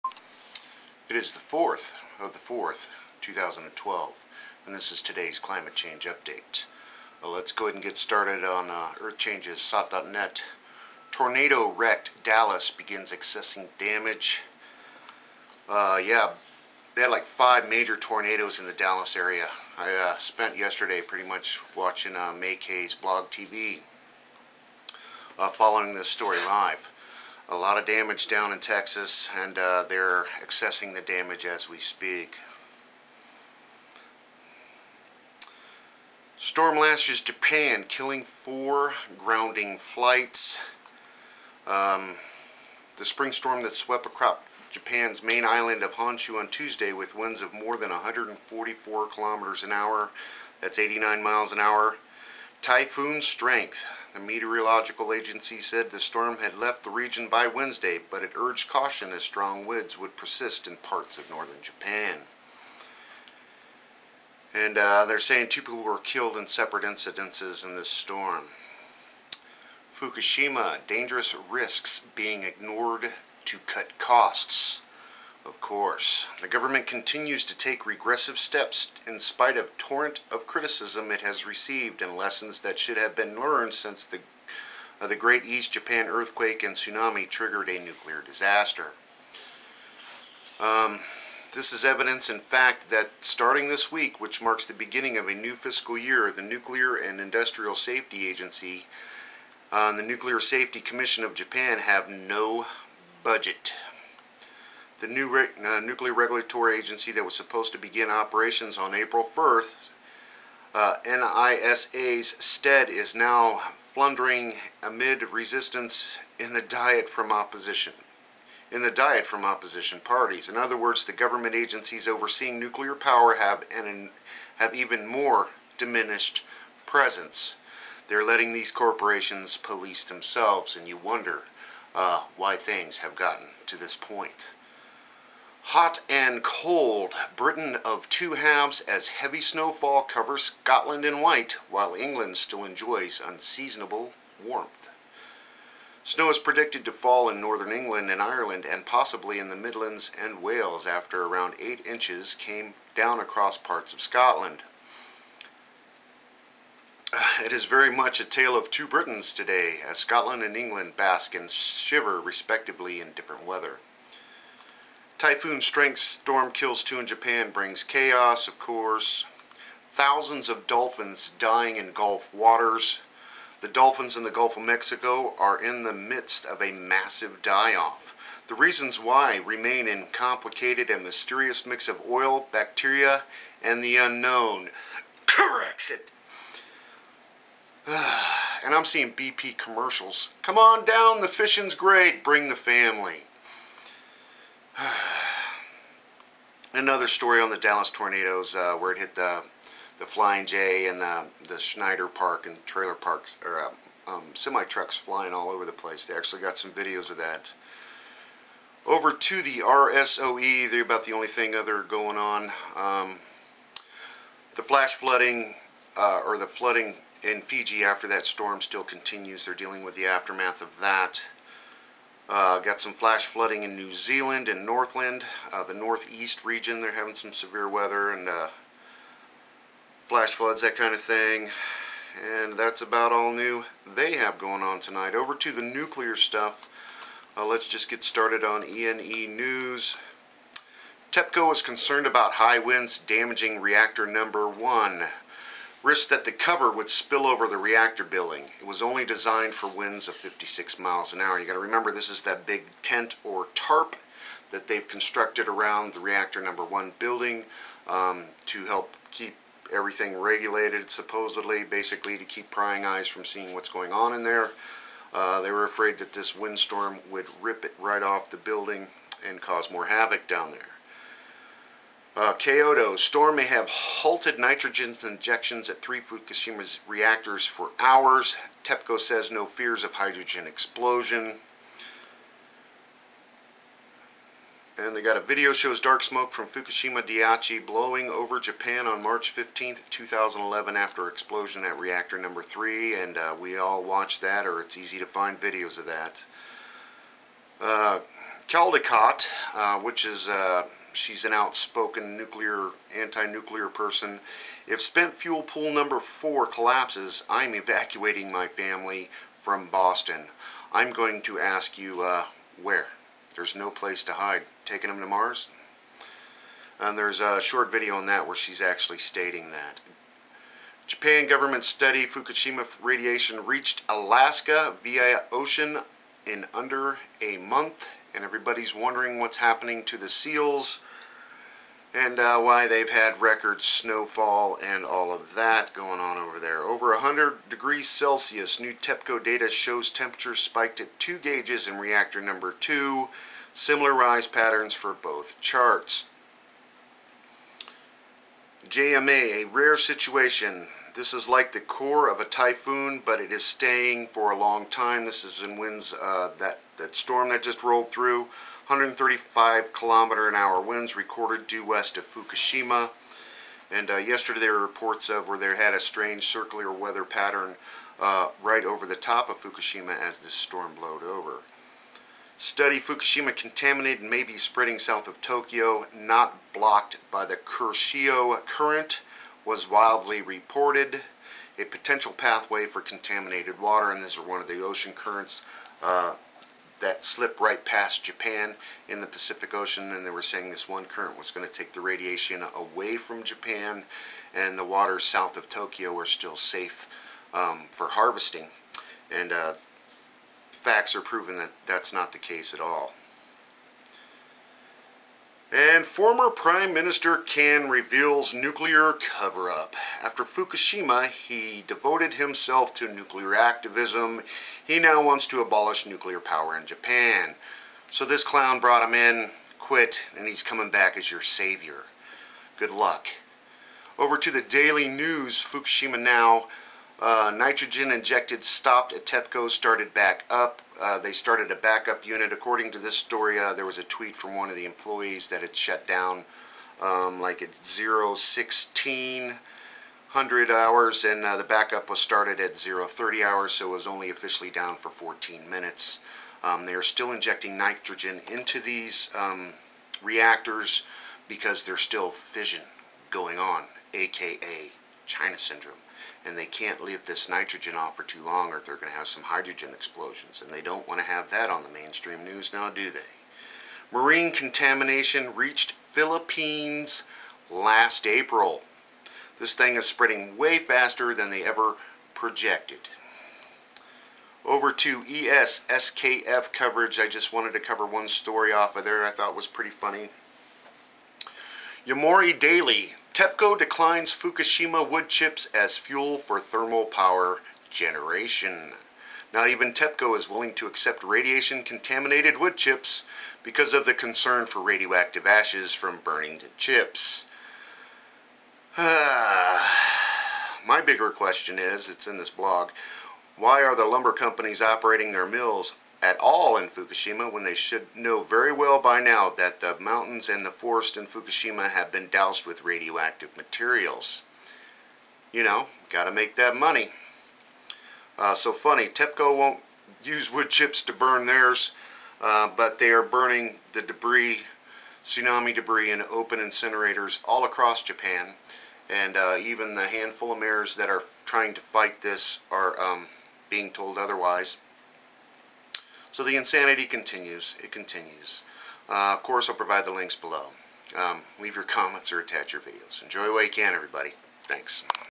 Climate Change Update (04 April 2012) Tornado-Wrecked Dallas
A destructive reminder of a young tornado season Wednesday left thousands without power and hundreds of homes pummeled or worse Wednesday, after the National Weather Service said as many as a dozen twisters touched down in a wrecking-ball swath of violent weather that stretched across Dallas and Fort Worth.

Kyodo: Storm may have halted nitrogen injections at three Fukushima reactors for hours — Tepco says no fears of hydrogen explosion

Not even TEPCO is willing to accept radiation-contaminated wood chips, because of the concern for radioactive ashes from burning the chips.